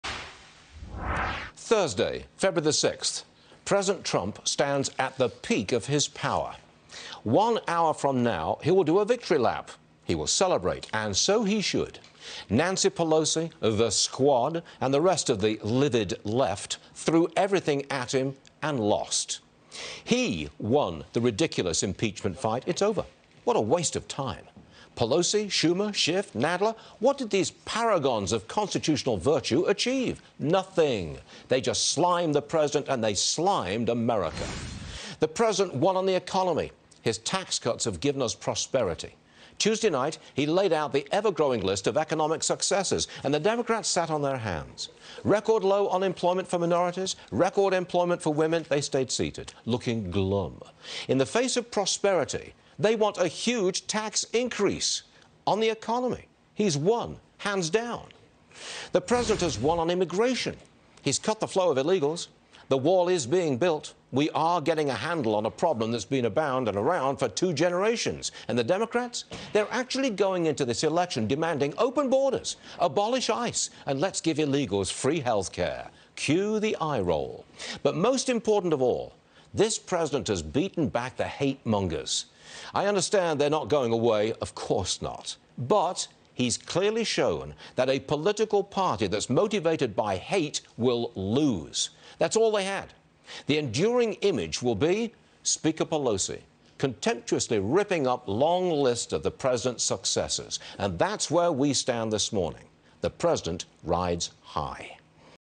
Varney: Trump has beaten back the hate-mongers
FOX Business’ Stuart Varney on President Trump’s victory on impeachment and the ‘glum’ faces of the Democrats.